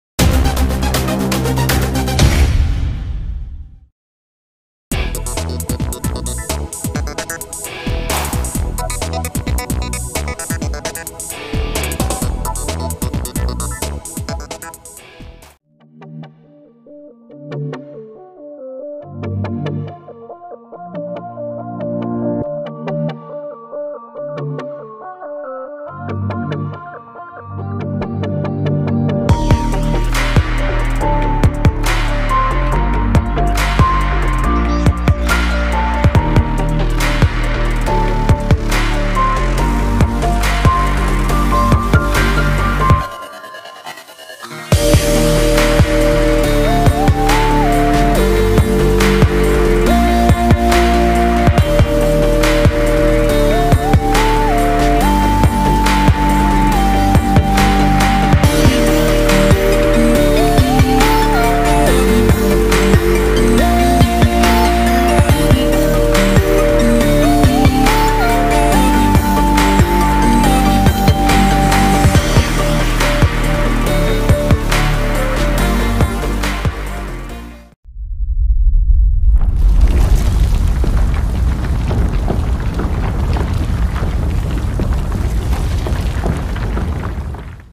xerox B405 Factory Reset - إعادة ضبط المصنع زيروكس
كيفية إعادة ظبط المصنع لموظيلات زيروكس الجيل الجديد versalink b406